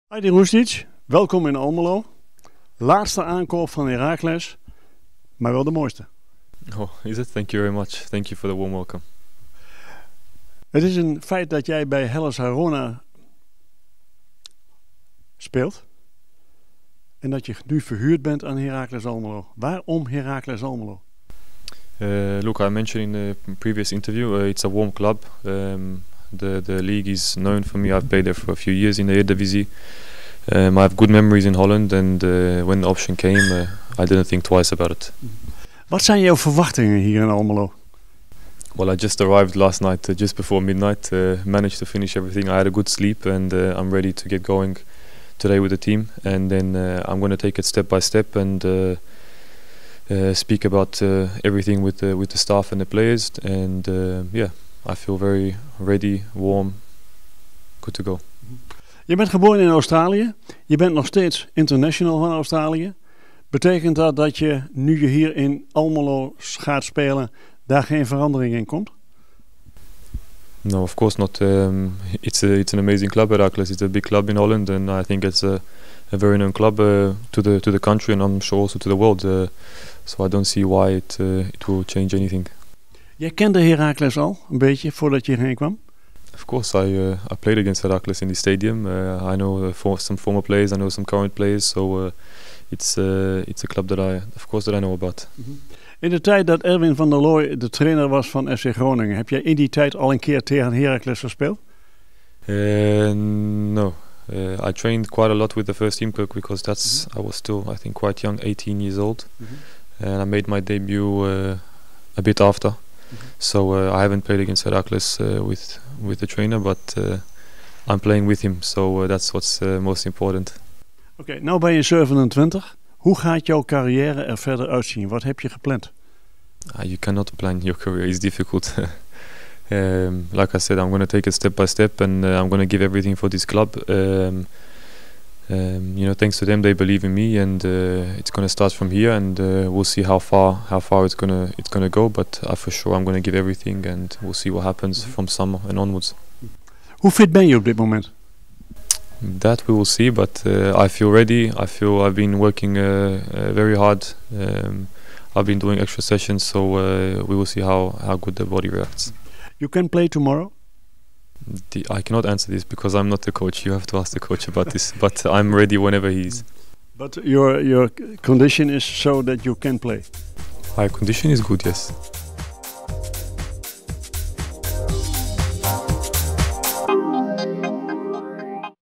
Interview met Hrustic
Vlak voor het einde van de transferperiode heeft Heracles zich versterkt met Ajdin Hrustić. De 27-jarige middenvelder komt over van Hellas Verona en tekent een contract tot de zomer van 2024.